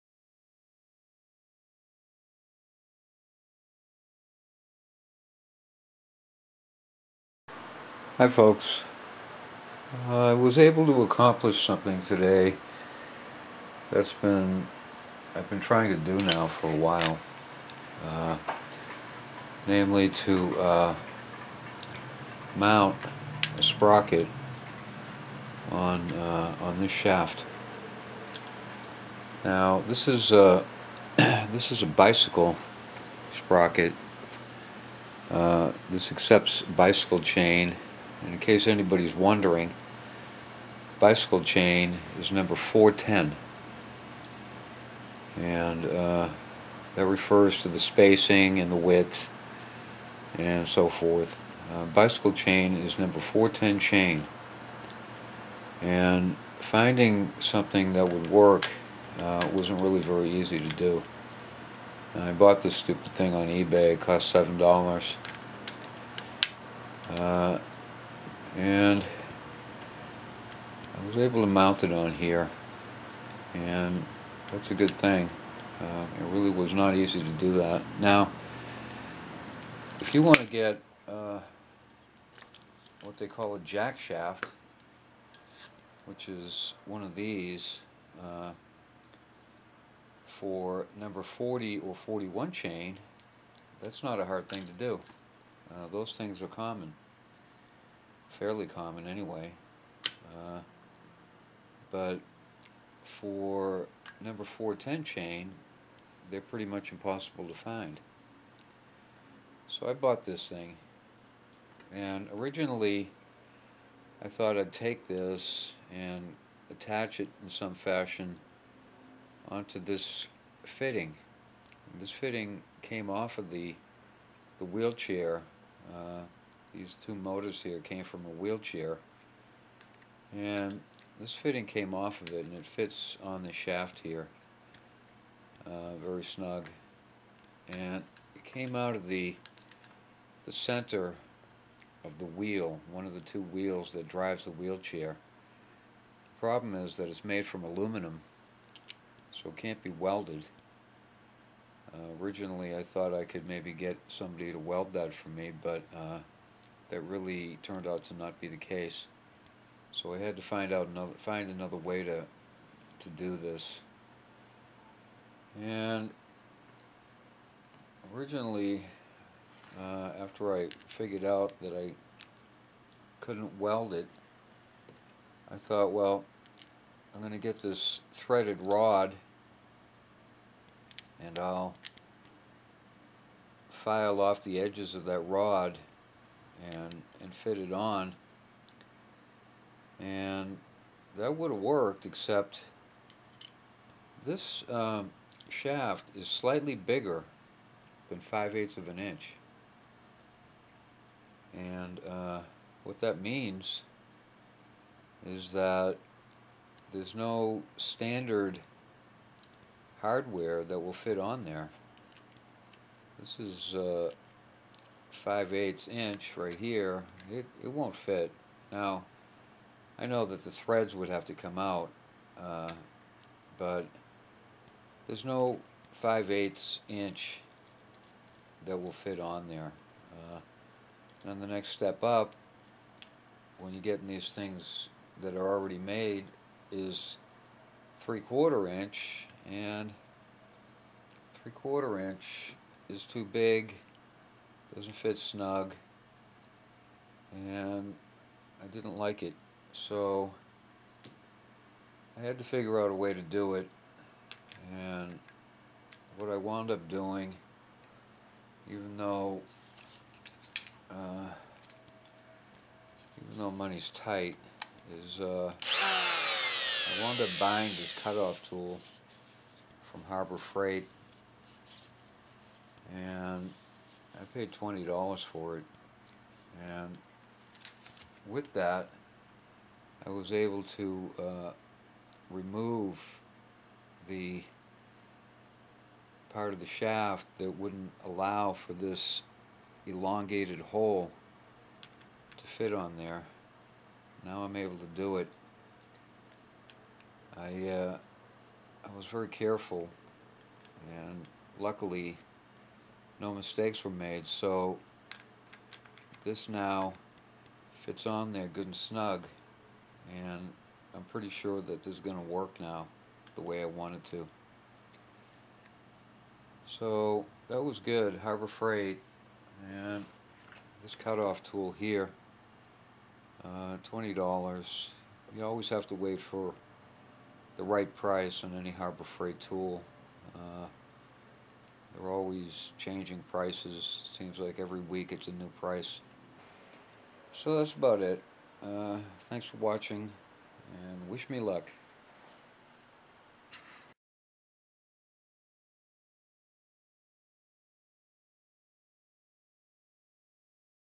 4 Wheel Electric Bicycle Car Progress 5
Harbor Freight cut-off tool saves the day. I was able to mount a sprocket onto the wheelchair motor drive shaft. Next step is mounting the motor and testing with the new motor controller.